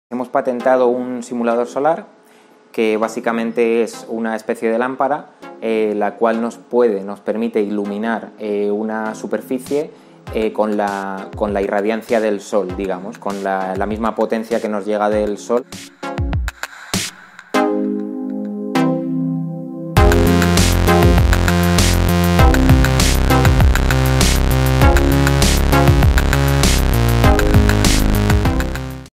SUNBOX: un simulador solar para la caracterización de fotodetectores y células solares | UC3M
Investigadores de la Universidad Carlos III de Madrid (UC3M) han registrado un modelo de simulador solar que permite iluminar una superficie con la misma irradiancia del sol, es decir, con su mismo rango espectral. Este sistema tiene aplicación en cualquier sector que necesite comprobar cómo reacciona un material a cierto tipo de luz.

Researchers at the Universidad Carlos III de Madrid (UC3M) have registered a solar simulator model that allows them to illuminate a surface with the same irradiance of the sun, in other words, with the same spectral range as the sun. This system can be used in any sector that needs to check how a material reacts to a certain type of light.